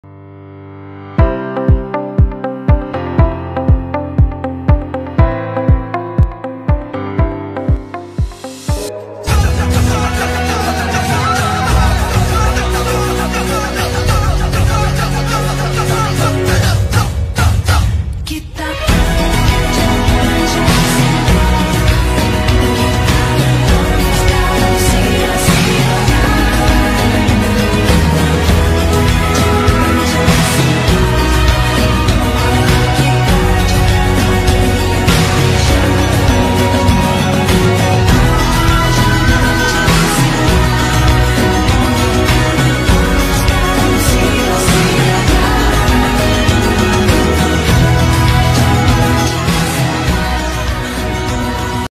Upacara Bendera | Satlantas Polres Situbondo
Sosialisasi keselamatan dan tata tertib berlalu lintas di sman 1 panarukan oleh satlantas polres situbondo